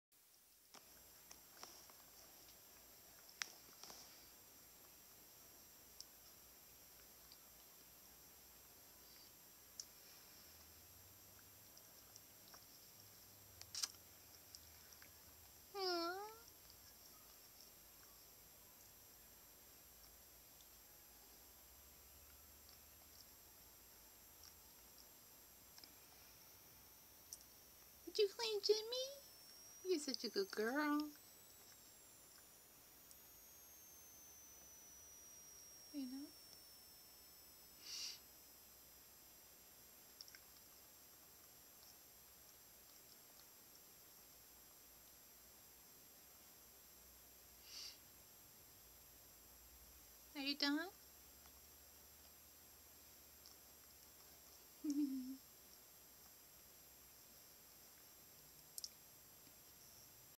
Kitty clean time
A short video of Sheldon grooming Jimmy.